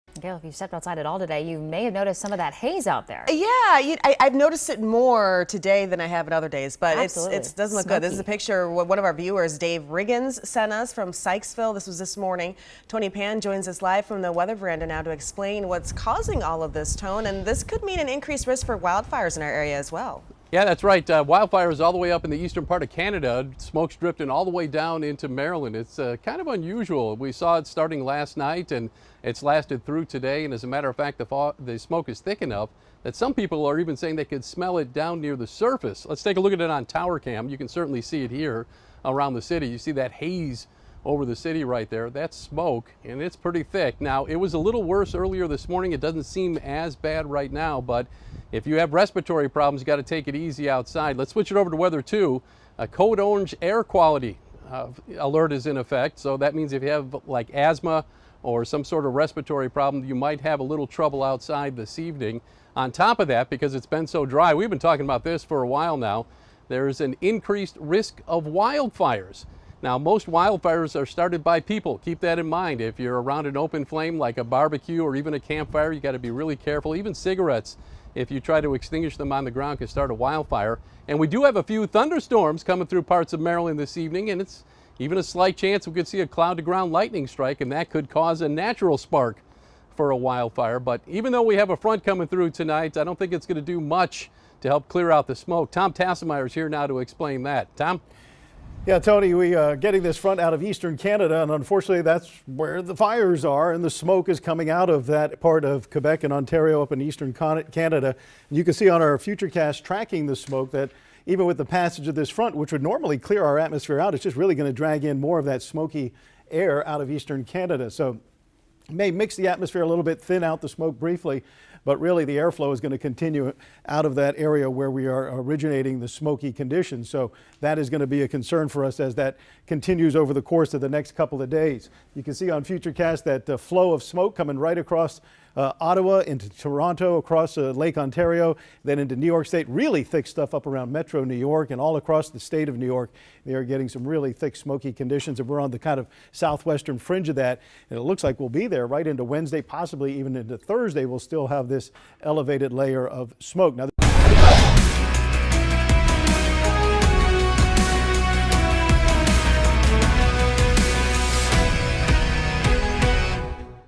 Weather Talk: Explaining the smoky haze over Maryland
Meteorologist Tony Pann explains the smoky haze seen over Maryland and talks potential ramifications.